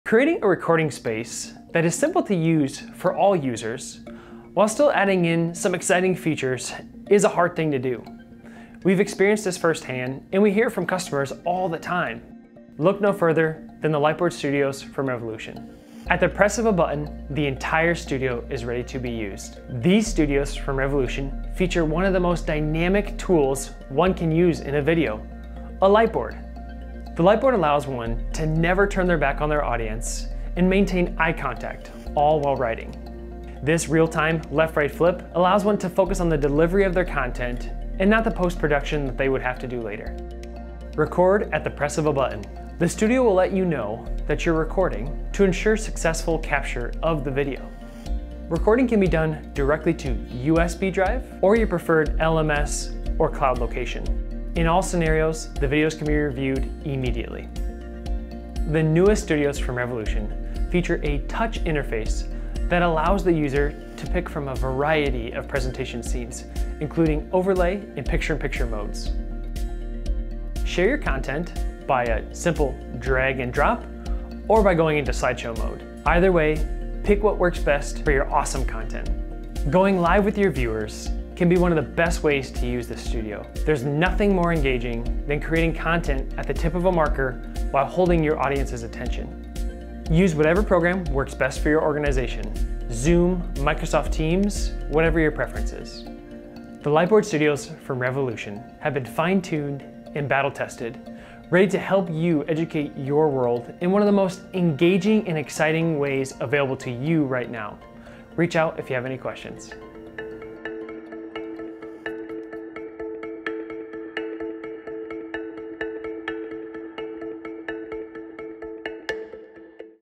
Our Newest Lightboard Studios | Revolution Lightboards | One Button to Record
Introducing our third-generation of Revolution Lightboard video-recording studio packages. These systems now include a simplified touchscreen control interface, multi-mode presentation options, an auxiliary input to capture graphics from a laptop, video recording data feedback and more!

Our patent-pending Lightboard Control Center™ technology allows for the intuitive addition of graphics into a variety of scene options in real-time without the need for any video editing. This is self-service video-recording at its finest. The whole system can be powered on and off with the press of a single button and users can be recording in seconds, with the press of a button. Go live on a video conferencing software of your choice or stream directly into any video management platform or learning management system of your choosing.